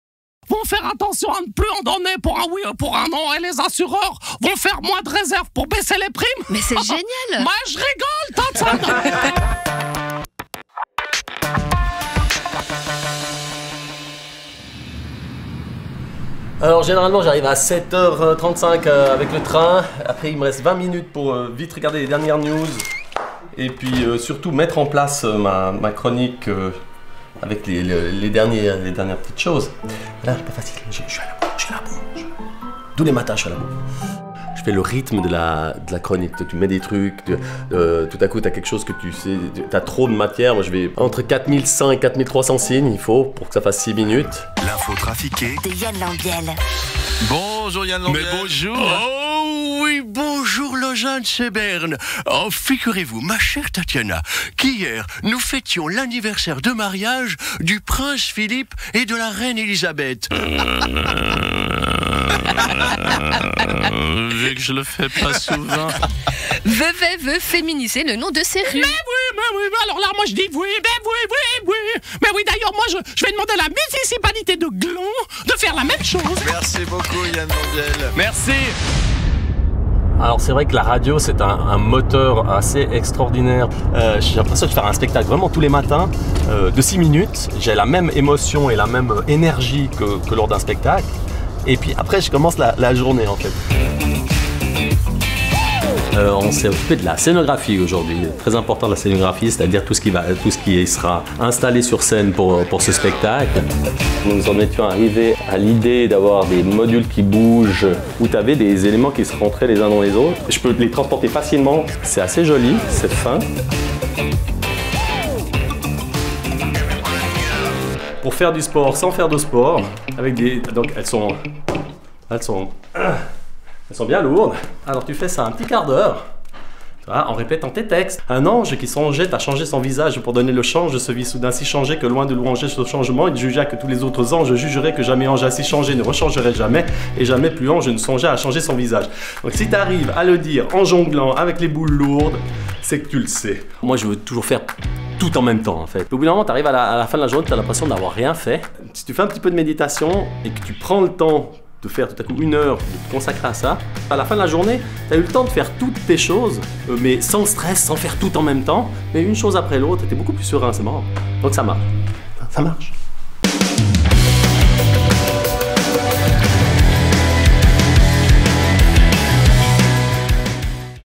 Yann Lambiel - Making of nouveau spectacle Multiple - Episode 3 Le Quotidien
Retrouvez le making of de tout le processus créatif de mon nouveau spectacle!

Réalisation: Take Off Productions
Production: Groupe E